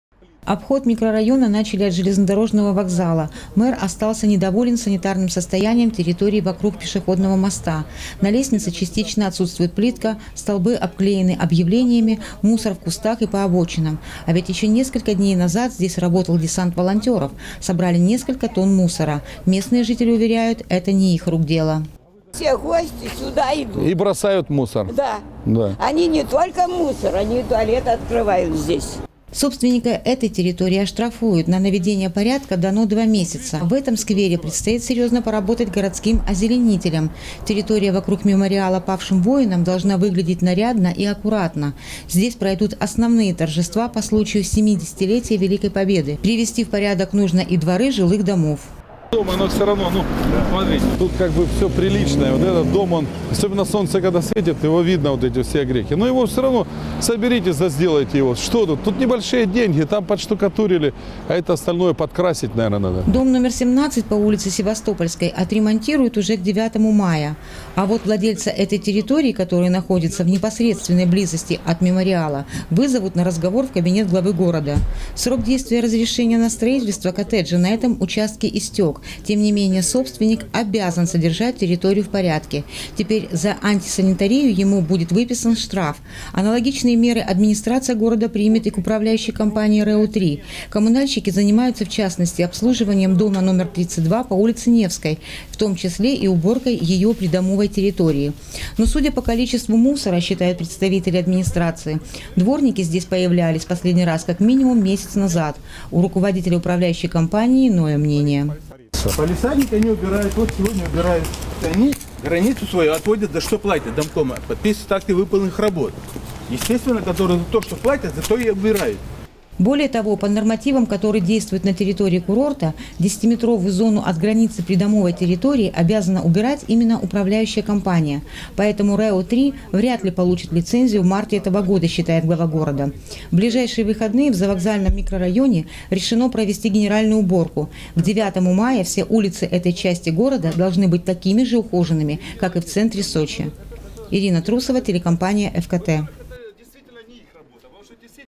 Управляющая компания РЭО 3 может лишиться лицензии. Новости 24 Сочи Эфкате
Участок улицы Параллельной, который находится за жилым комплексом «Остров мечты» в ближайшее время будет закрыт на реконструкцию. Об этом стало известно во время обхода главы города по микрорайону Завокзальный. Ремонт дороги будет выполнен в рамках наказов ветеранов Великой Отечественной войны.
Сочи. Новости.
Телеканал Эфкате.